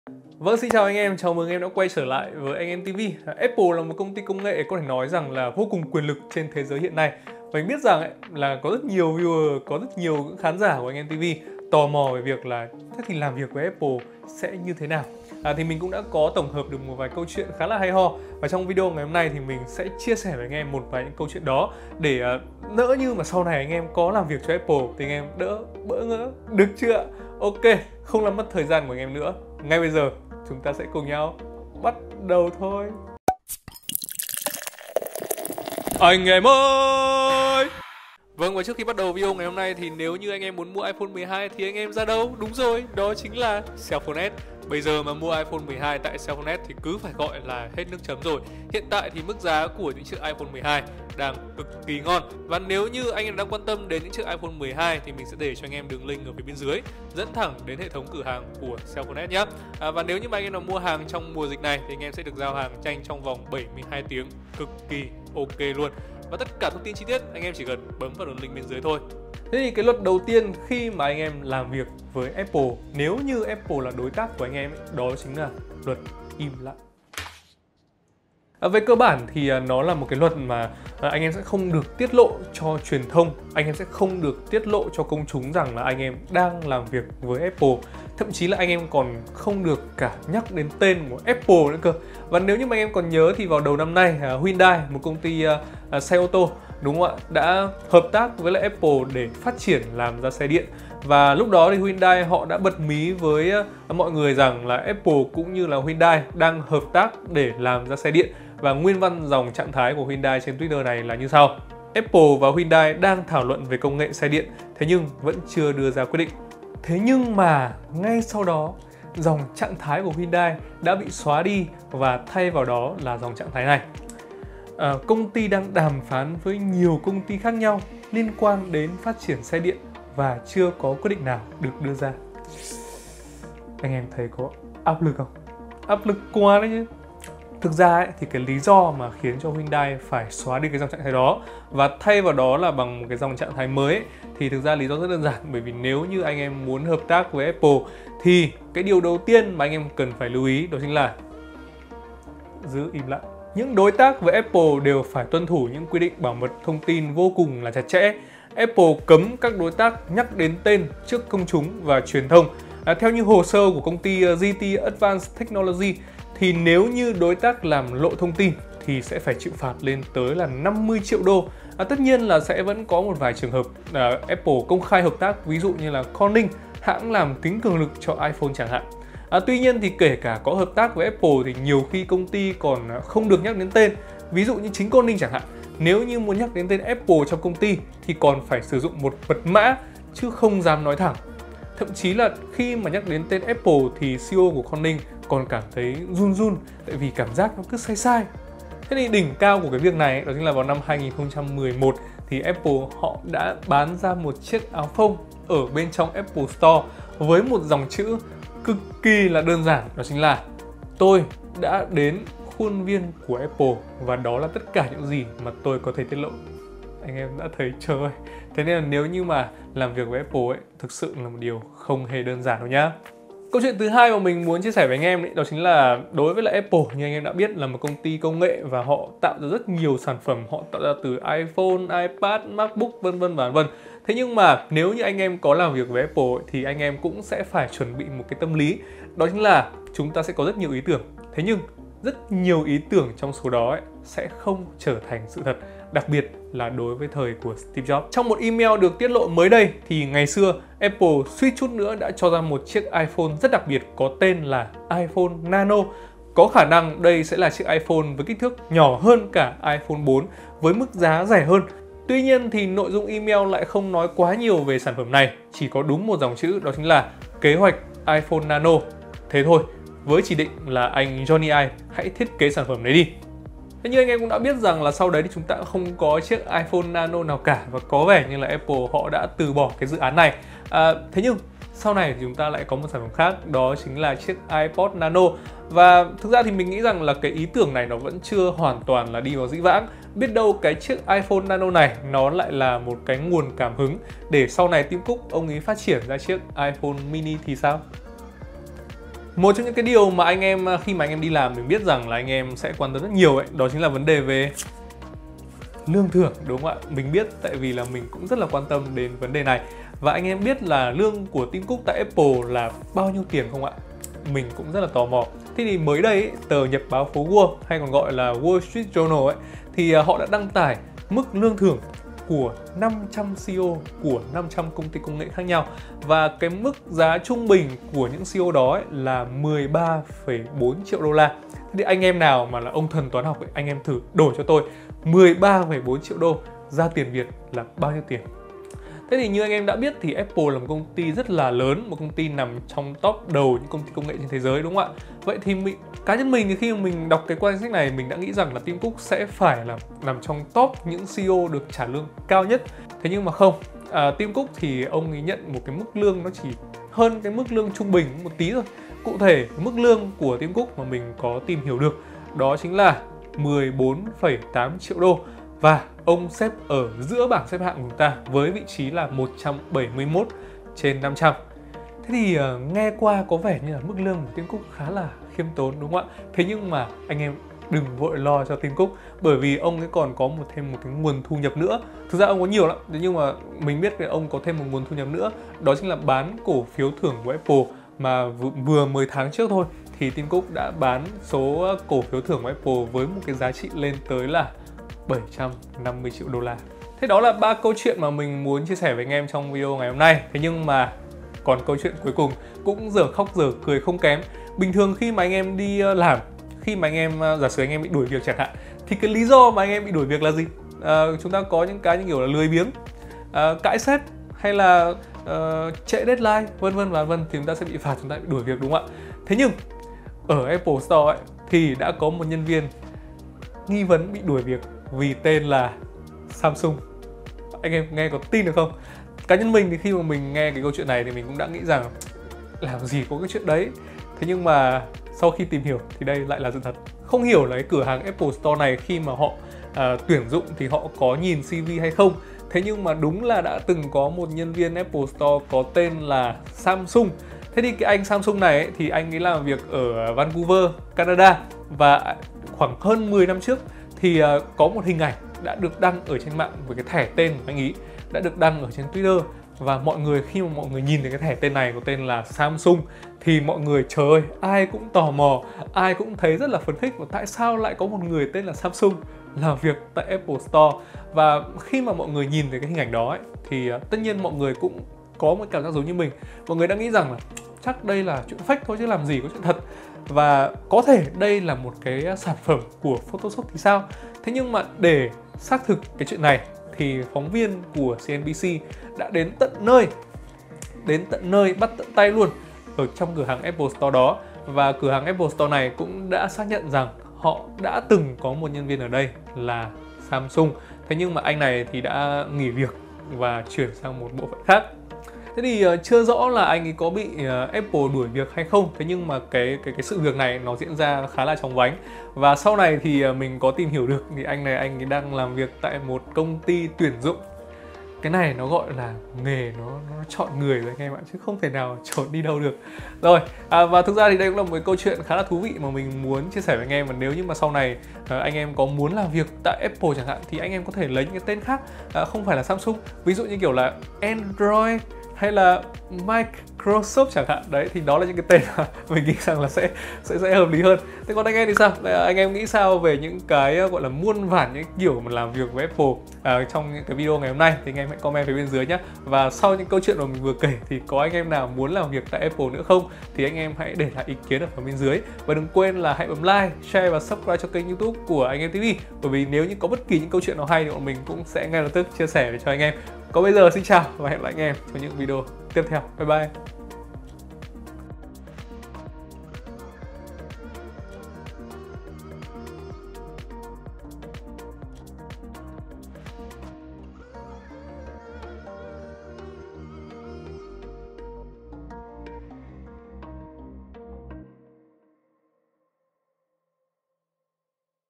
TIẾT LỘ MỨC LƯƠNG CỦA TIM COOK - LÀM VIỆC VỚI APPLE KHÓ NHƯ THẾ NÀO !??
Xin chào anh em, mình biết rằng có rất nhiều khán giả của kênh tò mò về việc làm việc với Apple thì sẽ như thế nào. Vậy thì trong video ngày hôm nay chúng ta hãy cùng nhau điểm qua một vài câu chuyện khi làm việc với Apple nhé. Thế nhưng xin nói trước rồi anh em, để làm việc được với Apple không hề dễ dàng, mà ngược lại còn vô cùng áp lực. Chi tiết như thế nào xin mời anh em xem video ngày hôm nay.

Còn nếu anhem nào muốn mua iPhone 12 thì bấm vào link này nha

Kết nối các thành viên AnhEmTV
Duy Thẩm: